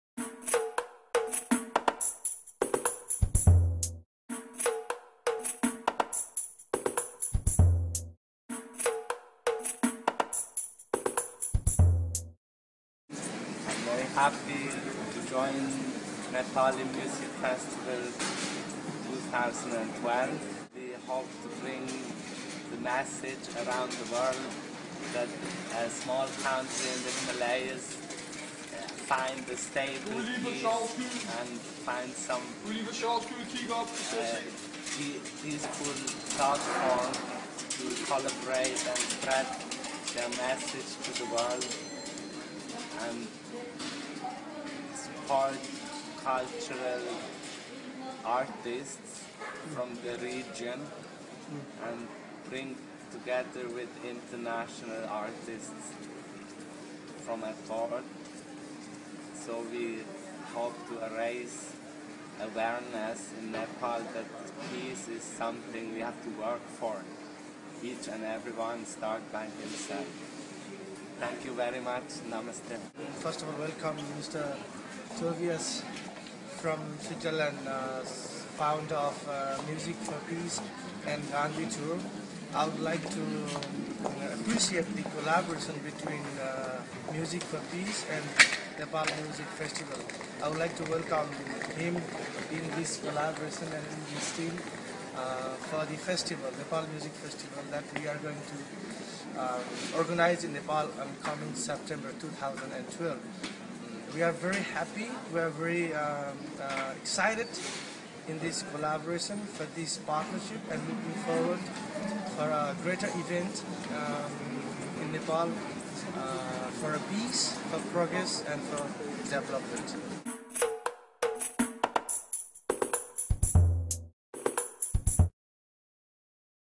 Music 4 Peace / Nepal Music Festival
Nepal Music Festival partners up with Music 4 Peace to organize a Music Festival for Peace and Progress in Nepal in 2012
Ranjan Ojha founder of Nepal Music Festival meets Tobias Huber founder of Music 4 Peace in Copenhagen in 2012